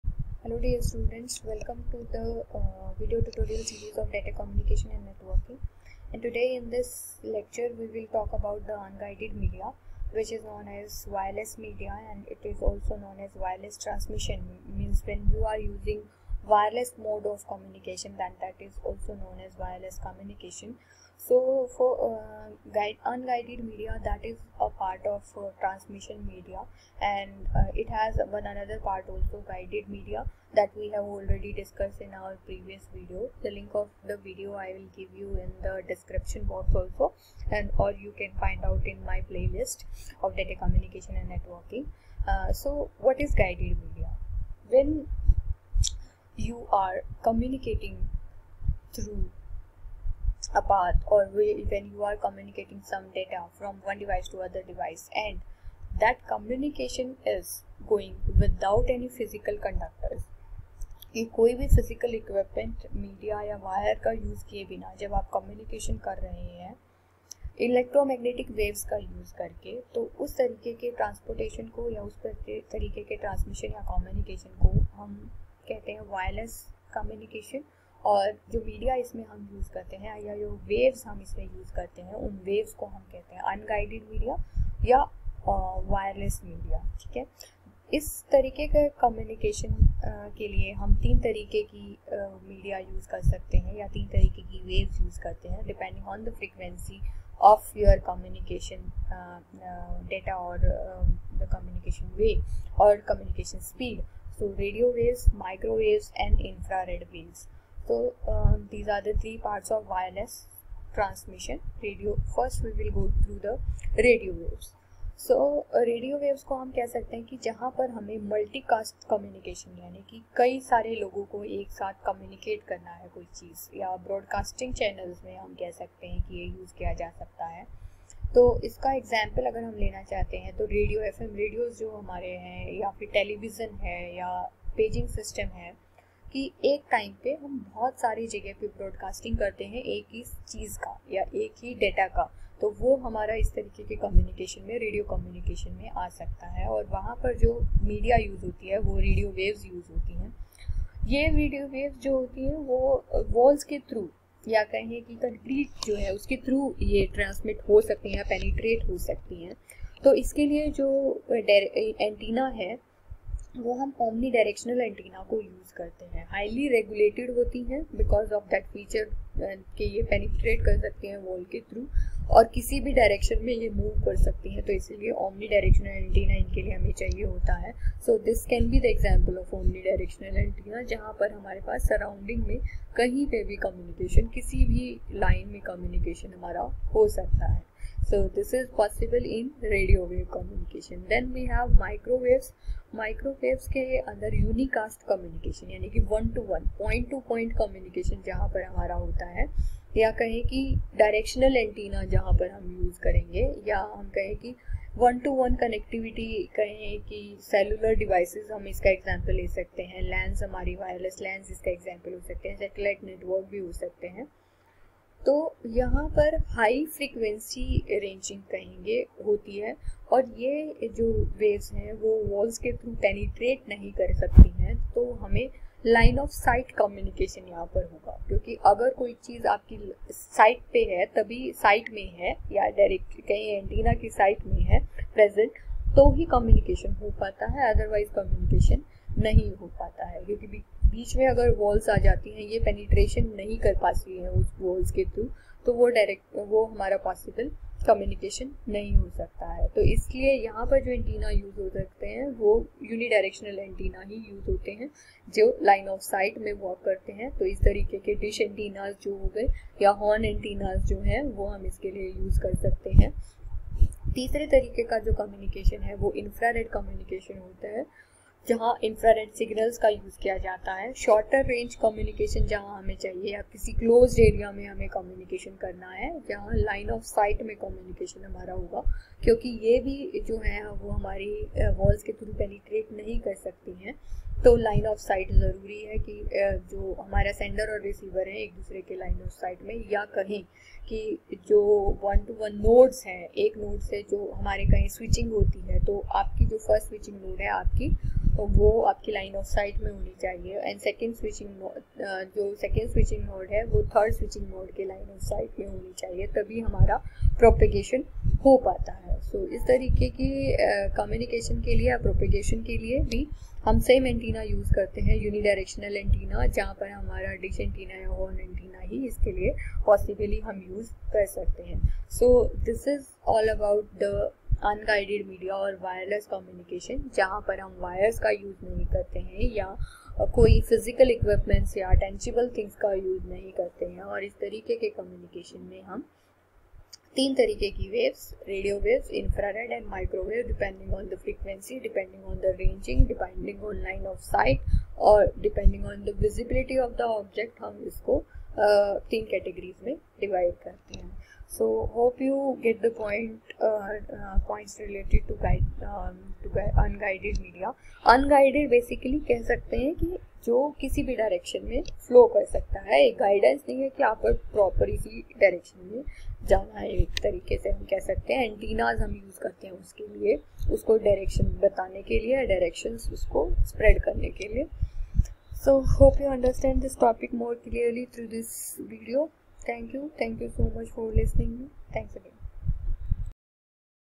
Unguided Media
This video explain about the unguided media or wireless media. It also explain about wireless communication.